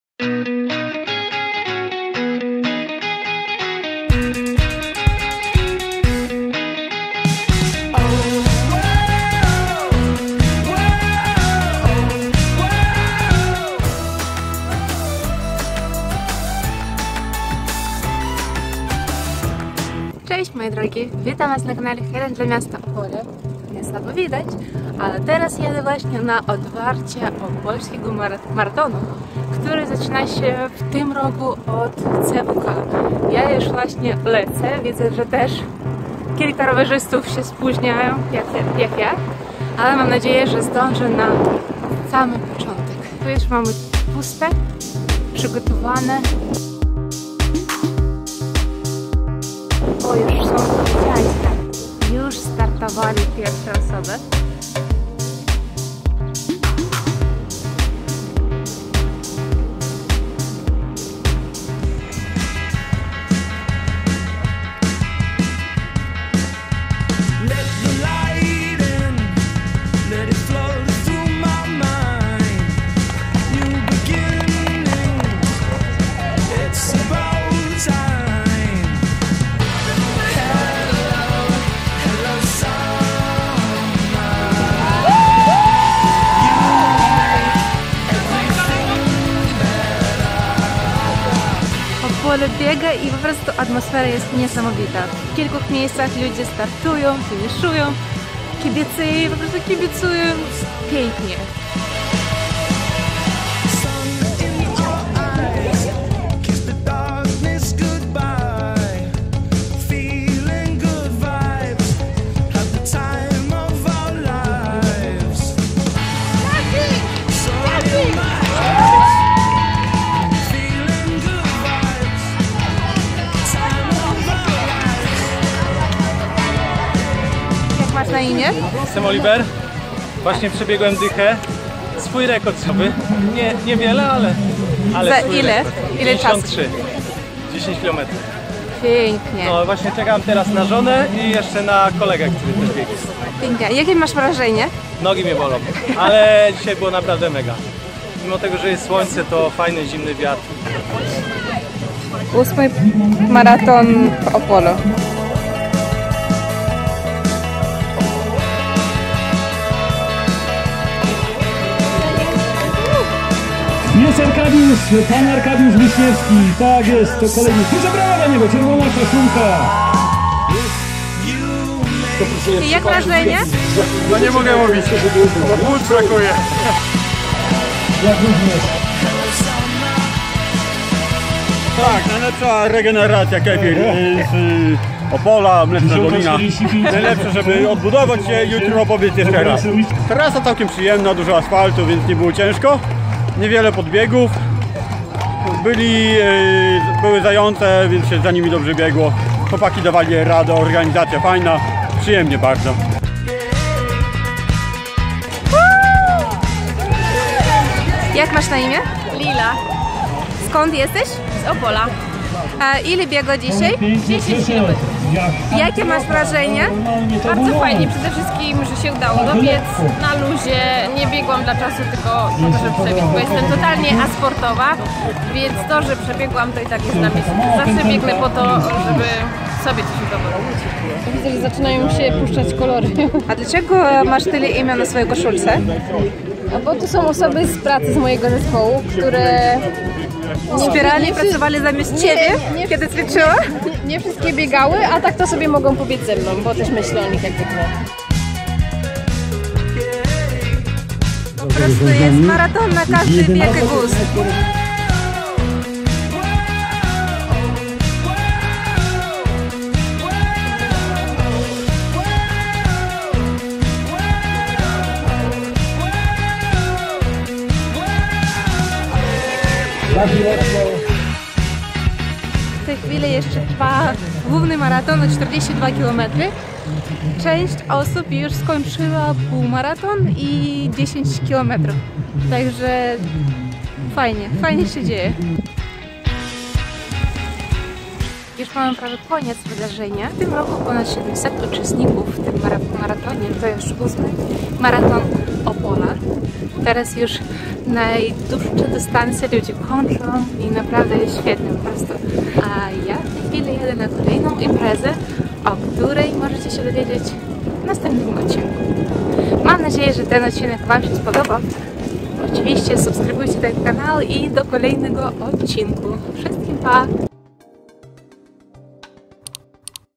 Opole biega!
W Opolu się odbył 8 Maraton!